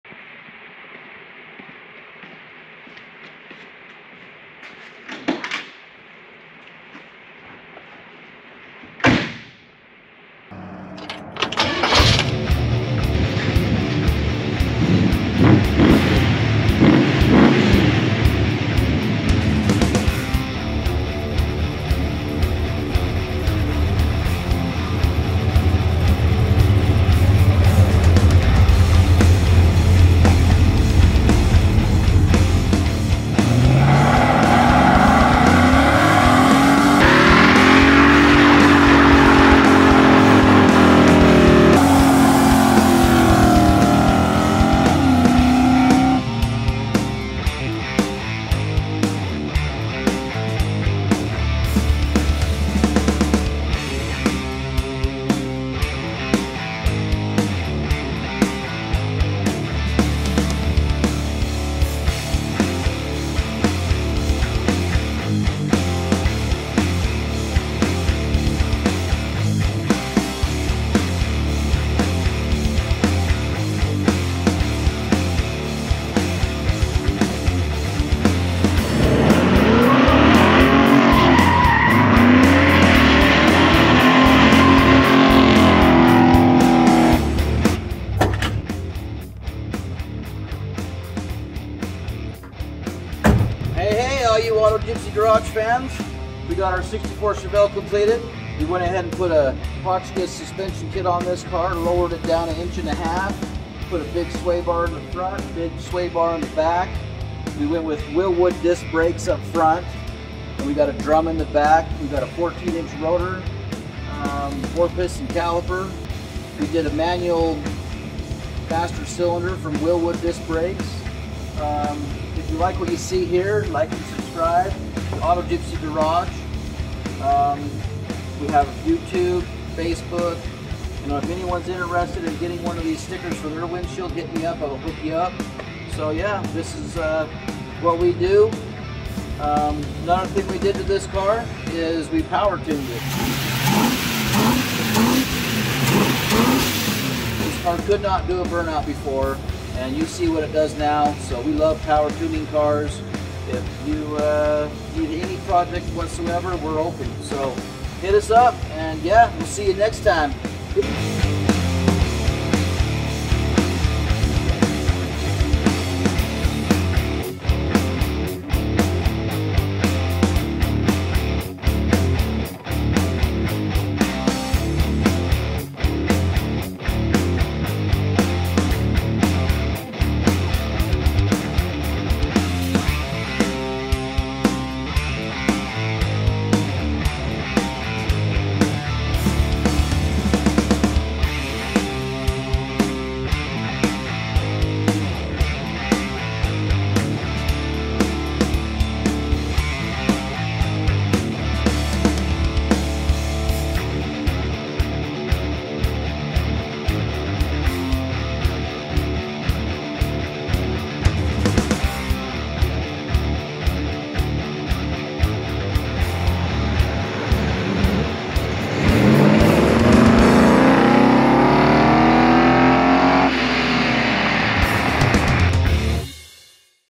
1964 Chevelle gets Hotchkis suspension & Willwood disk brake upgrade
Join Auto Gypsy Garage as we say good bye after testing out the new Hotchkis suspension and Willwood disk brakes on the 1964 Chevelle project.